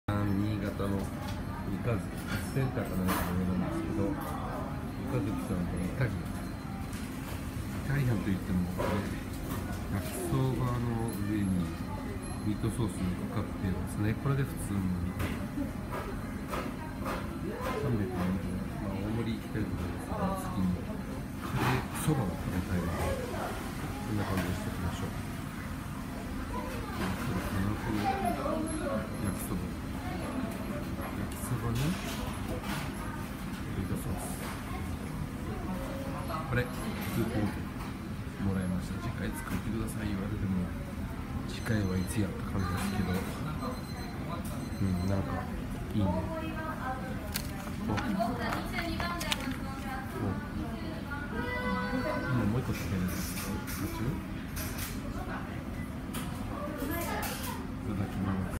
【イタリアン】 新潟B級グルメ みかづきバスセンター2階で 焼きそばにミートソースの 【新潟ソウルフード】 激安美味しいケンミンSHOW#イタリアン#新潟#ソウルフード#みかづき
【イタリアン】 新潟B級グルメ みかづきバスセンター2階で 焼きそばにミートソースの  【新潟ソウルフード】 激安美味しいケンミンSHOWで 紹介の 旨旨飯  20180603